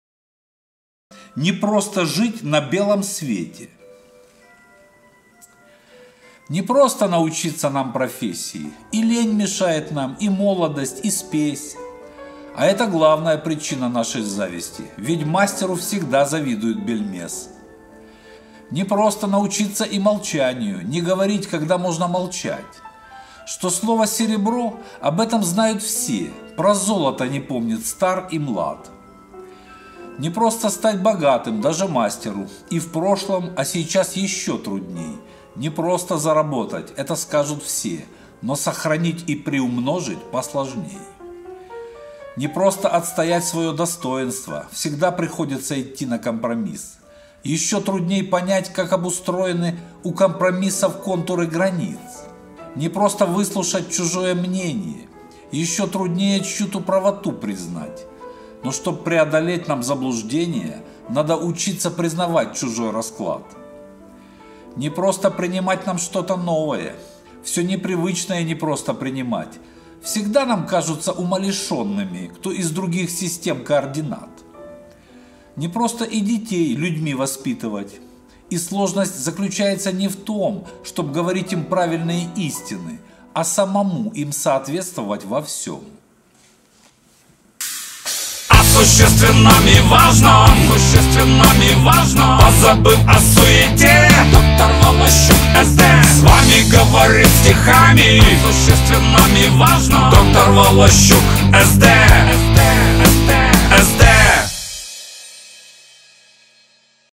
Непросто жить на белом свете
Непросто научиться нам профессии,
И лень мешает нам, и молодость, и спесь.
А это главная причина нашей зависти,
Ведь, мастеру всегда завидует бельмес.

Непросто научиться и молчанию,
Не говорить, когда можно молчать.
Что слово серебро об этом знают все,
Про золото не помнит стар и млад.

Непросто стать богатым, даже мастеру,
И в прошлом, а сейчас ещё трудней.
Непросто заработать – это скажут все,
Но сохранить и приумножить посложней.

Непросто отстоять своё достоинство,
Всегда приходится идти на компромисс.
Ещё трудней понять, как обустроены
У компромиссов контуры границ.

Непросто выслушать чужое мнение,
Ещё труднее чью-то правоту признать.
Но, чтоб преодолеть нам заблуждения,
Надо учиться признавать чужой расклад.

Непросто принимать нам что-то новое,
Всё непривычное непросто принимать.
Всегда нам кажутся умалишёнными,
Кто из других систем координат.

Непросто и детей людьми воспитывать,
И сложность заключается не в том,
Чтоб говорить им правильные истины,
А самому им соответствовать во всём.